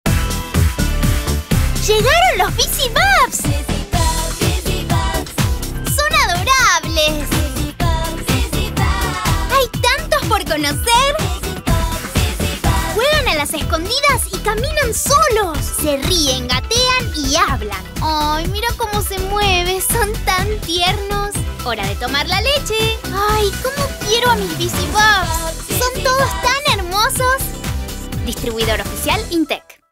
¡Llegaron los Bizzy Bubs!
¡Son unos adorables bebés! Juegan a las escondidas, caminan solos, se rien, gatean y hablan. ¡Son tan tiernos!